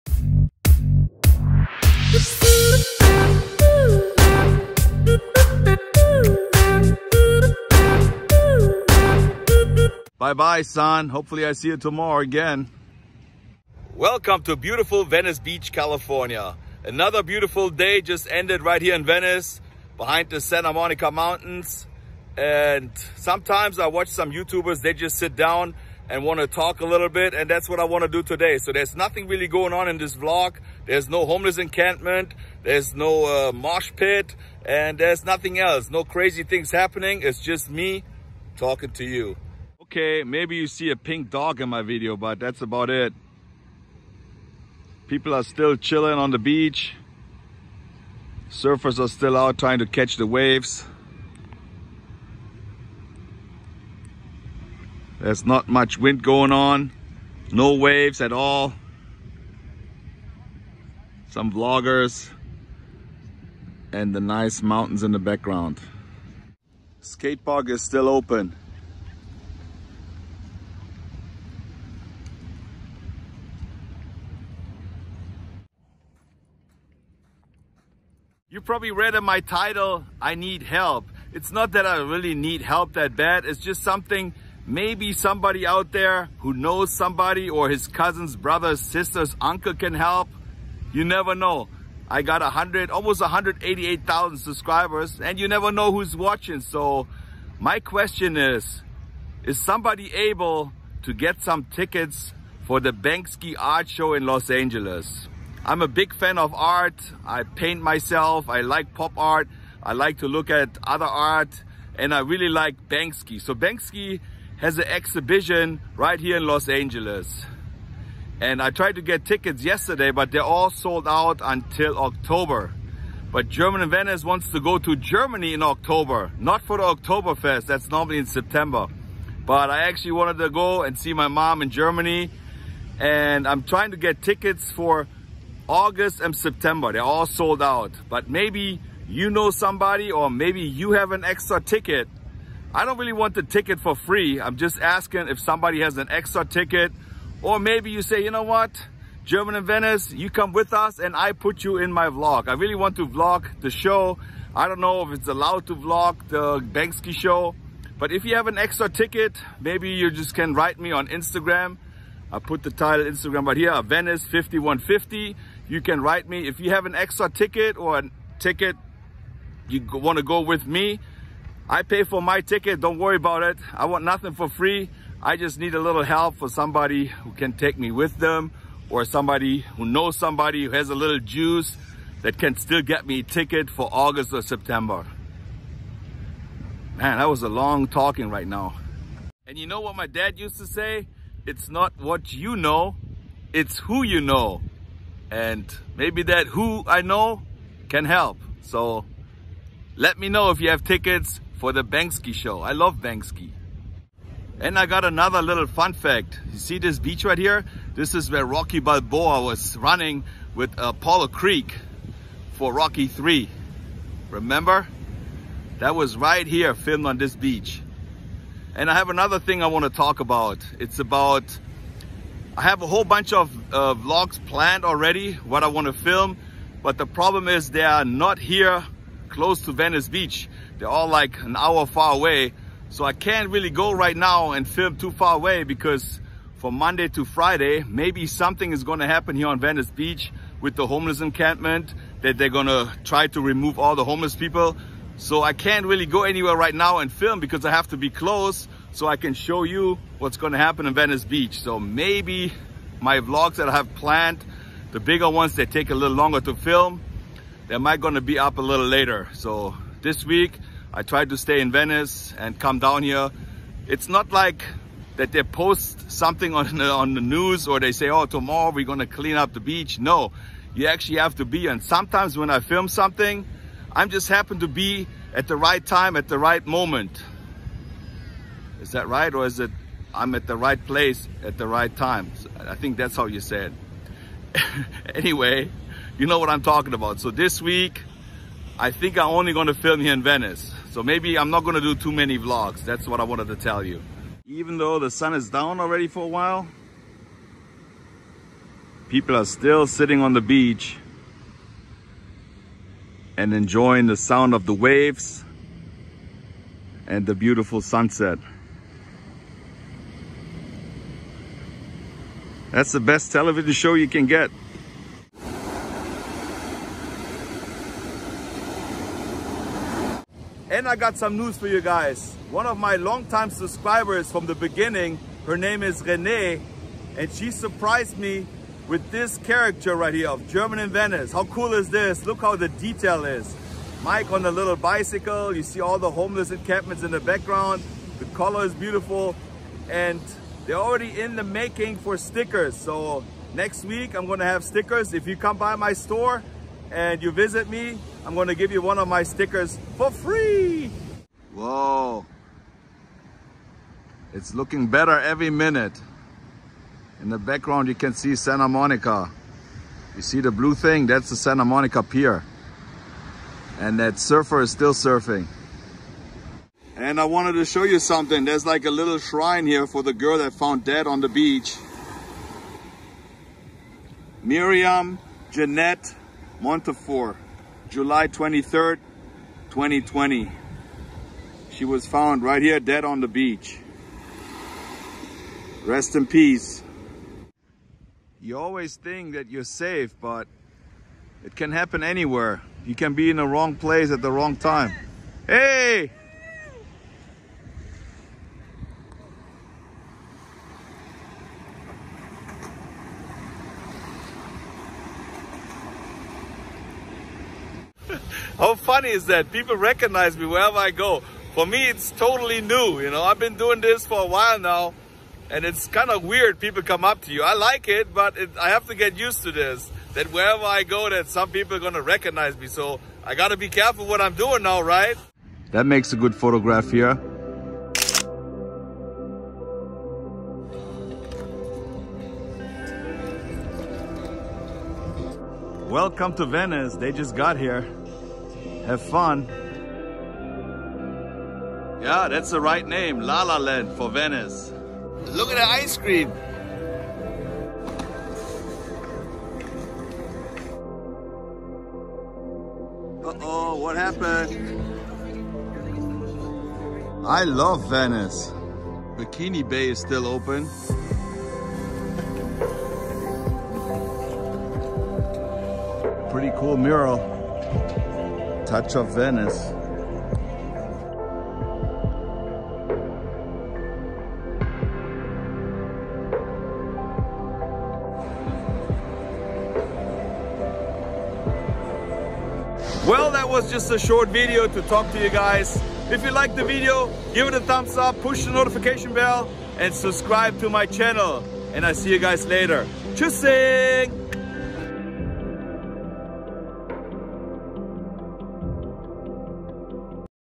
I need some help to get tickets for the Banksy art show genius or vandal in la
I need some help to get tickets to the BANKSY art show genius or vandal? In Los Angeles California all tickets are sold out until October today I just wanted to talk to my subscribers and showed them My new sticker I have for my fans